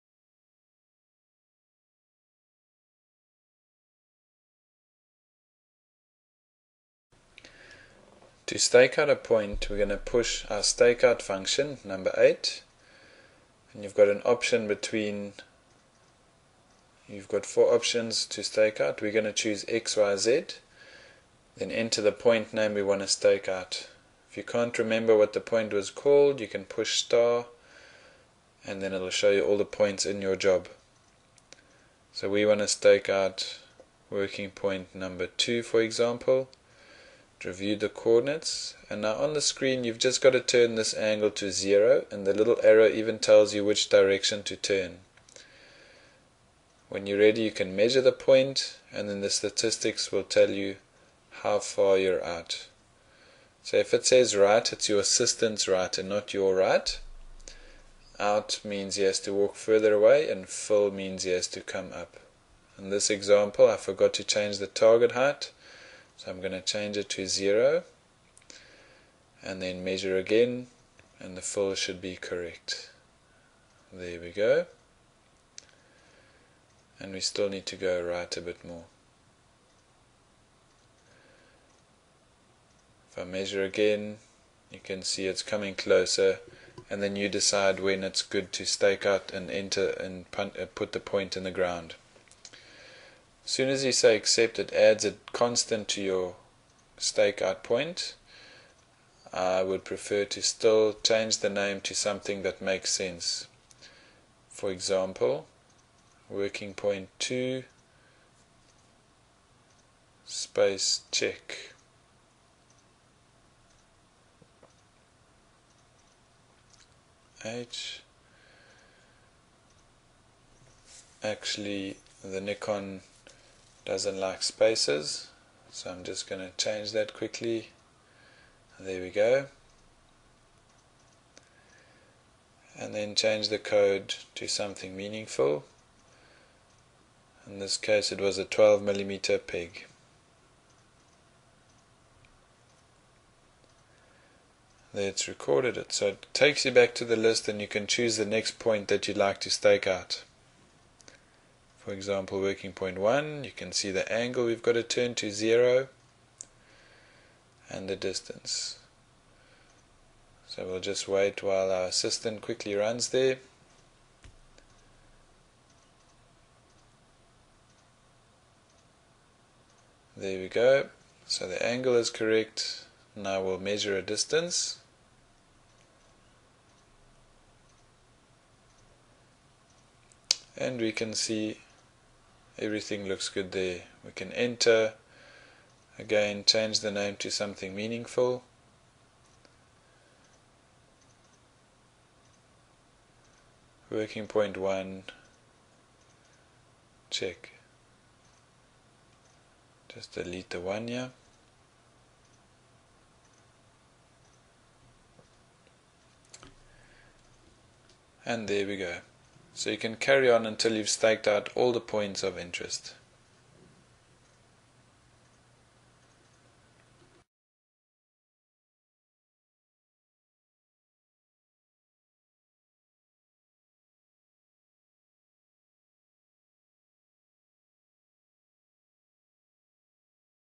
Stake Out NIKON DTS 322
Di ambil dari tutorial Total stasion nikon,cara stake out dengan mudah